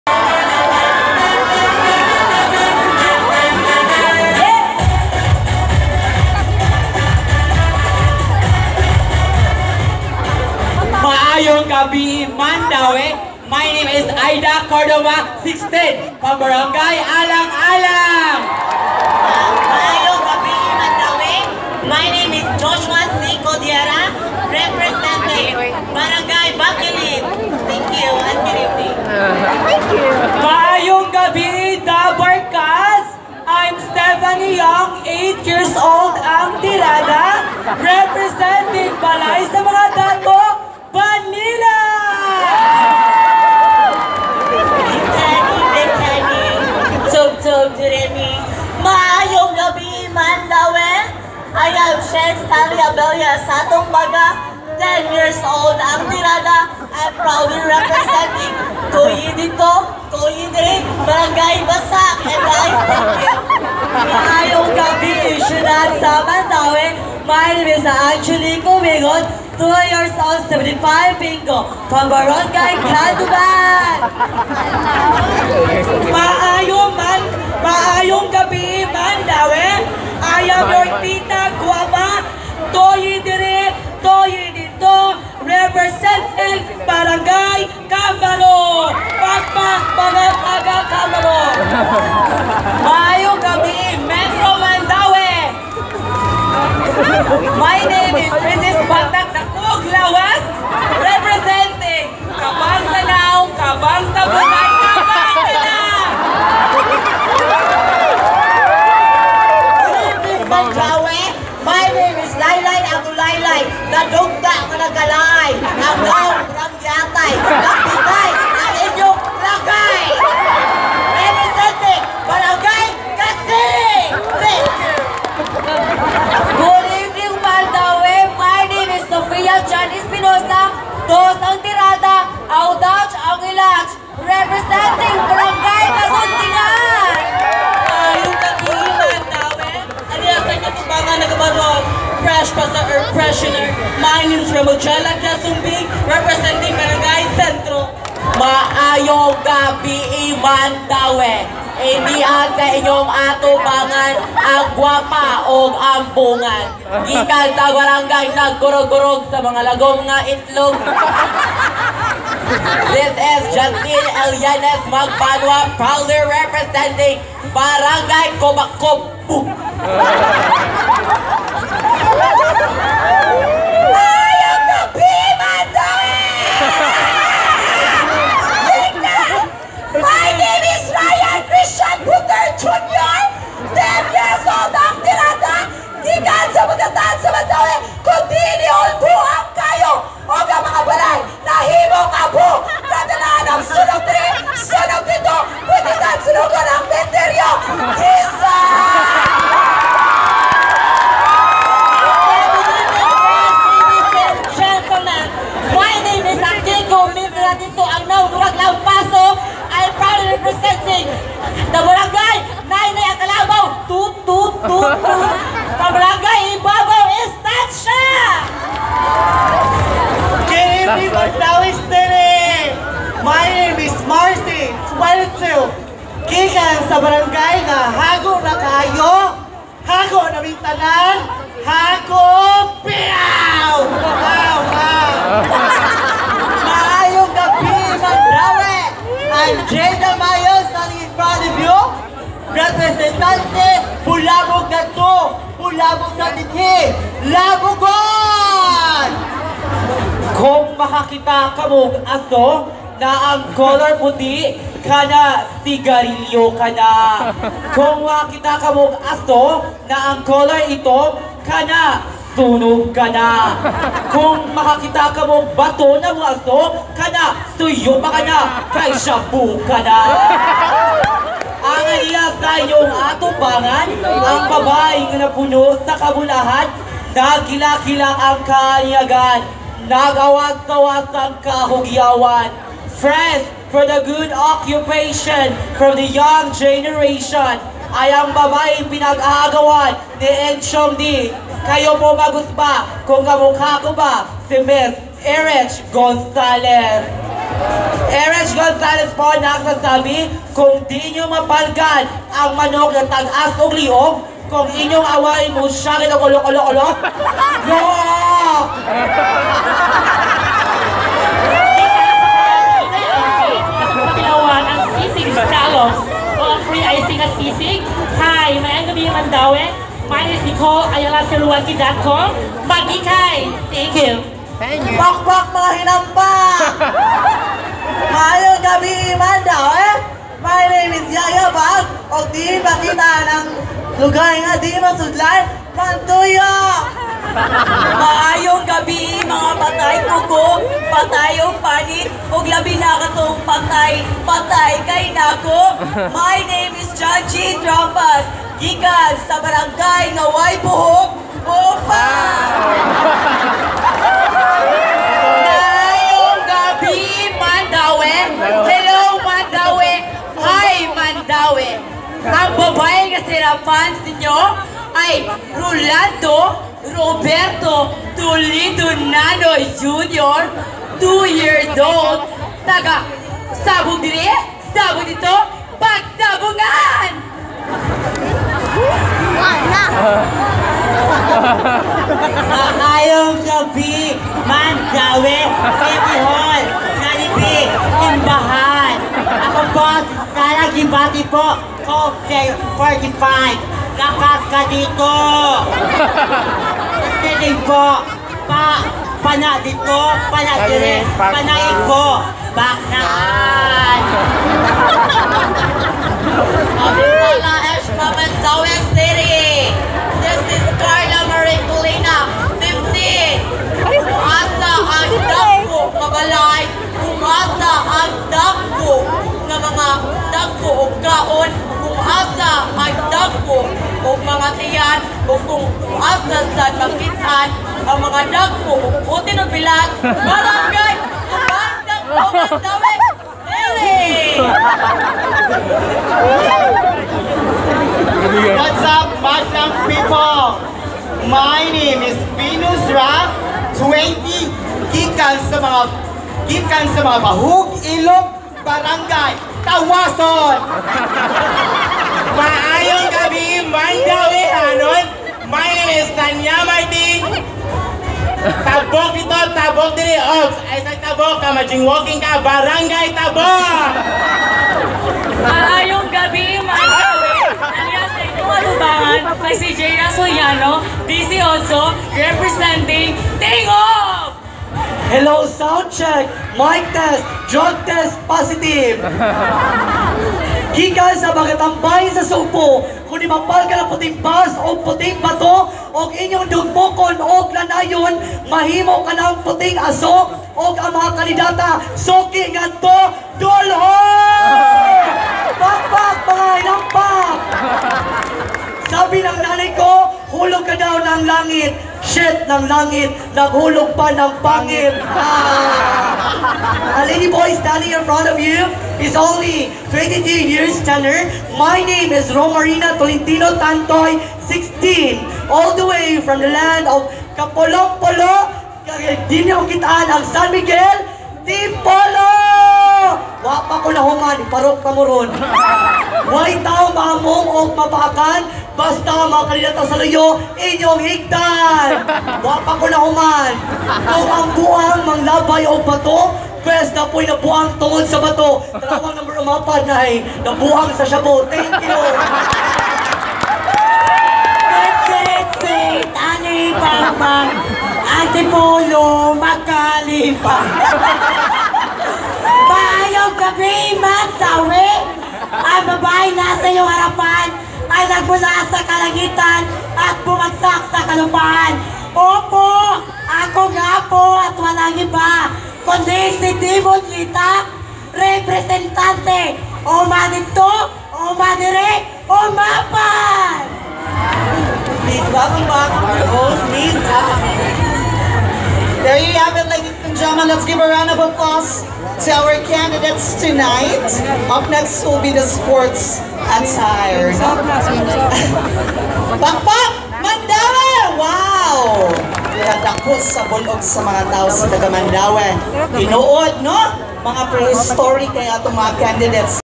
Miss Gay Jurassic Mandaue 2018 - Introduction 2
Miss Gay Jurassic Mandaue is part of Mandaue's Mid-year Cultural Summit 2018 held at the Heritage Plaza on May 5. The show was all about fun and laughter and brought great entertainment to the huge crowd.